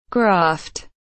How to pronounce Graphed / Graphed pronunciation
Raccoon teaches you how to pronounce Graphed . Raccoon vous apprends a prononcer Graphed . Como pronunciar Graphed